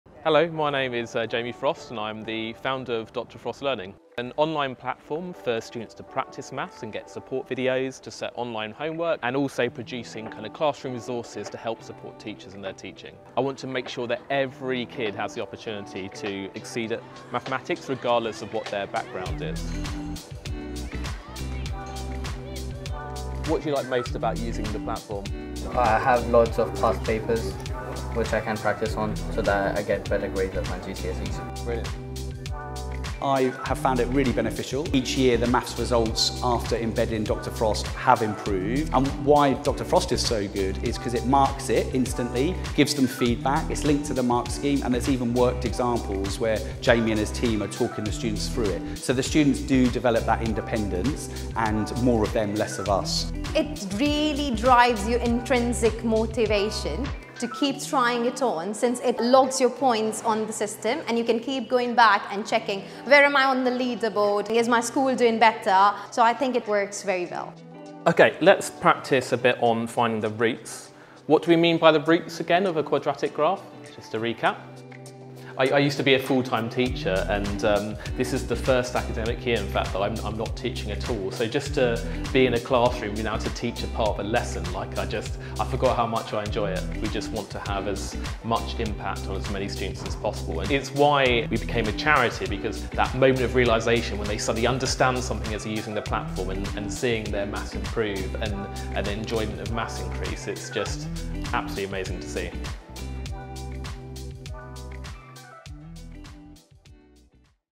Meet Dr. Frost: The Maths Charity Helping Schools Worldwide
Meet Dr. Jamie Frost, a British maths educator, who founded Dr Frost Learning, a charity dedicated to providing a high-quality education for all.

He created DrFrostMaths, an online platform offering free teaching and learning resources globally.

In 2020, he received the Global Teacher Prize 'Covid Hero Award' for supporting schools during lockdowns. Dr Frost Learning aims to address social inequality by offering resources for teaching mathematics and other subjects, along with training for educators.

Dr Frost Learning's mission is to provide high-quality education for all, regardless of income, based on the philosophy that education is a fundamental right essential to addressing social inequality globally.